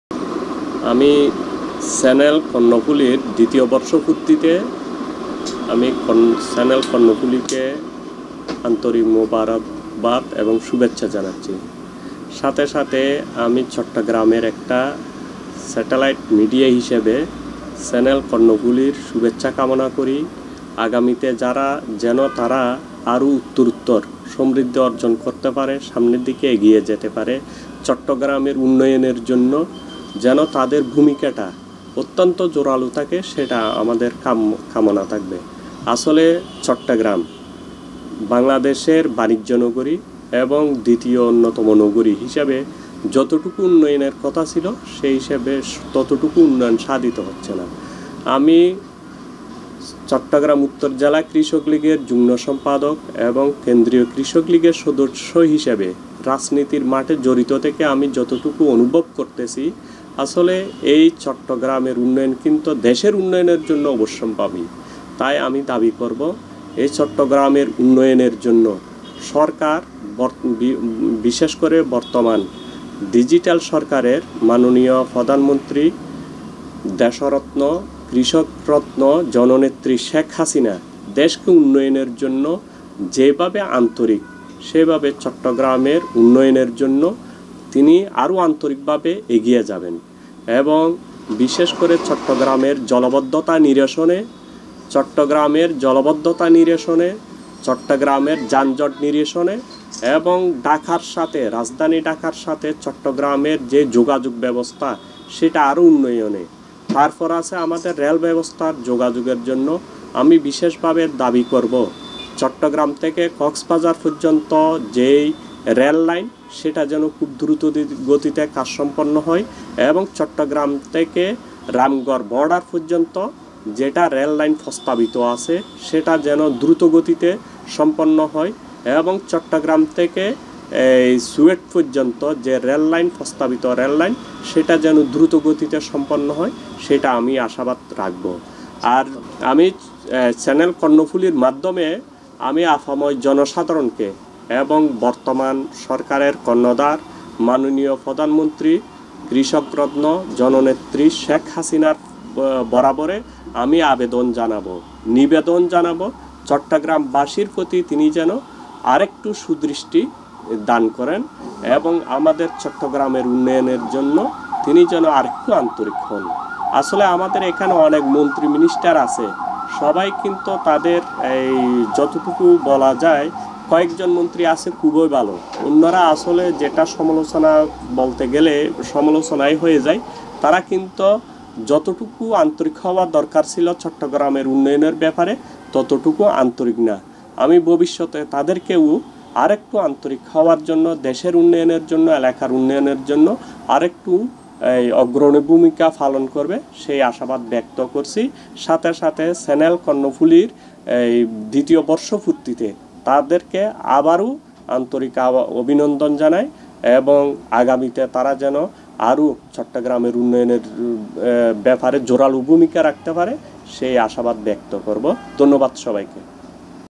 কর্ণফুলীর ২য় বর্ষপূর্তিতে শুভেচ্ছা জানালেন চট্টগ্রাম উত্তর কৃষকলীগ এর যুগ্ম সম্পাদক, ফয়েজুল ইসলাম
কর্ণফুলীর ২য় বর্ষপূর্তিতে শুভেচ্ছা জানালেন চট্টগ্রাম উত্তর জেলা কৃষকলীগ এর যুগ্ম সম্পাদক, মোঃ ফয়েজুল ইসলাম (আই.টি.পি)